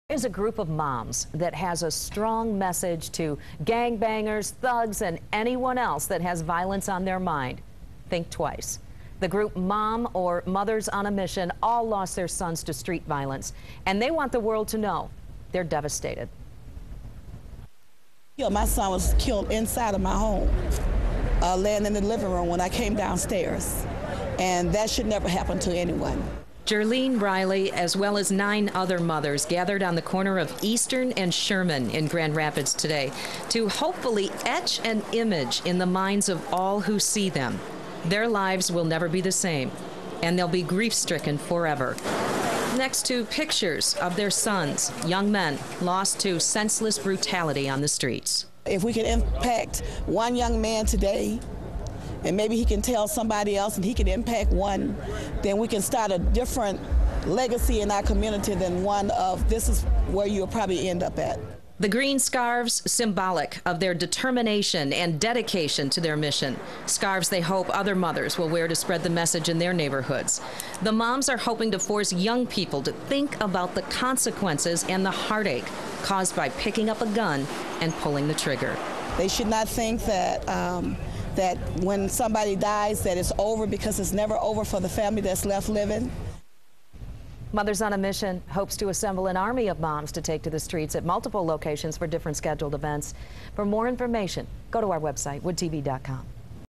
"Moms" hope to end GR street violence
There's a group of moms that have a strong message to think twice before committing street violence.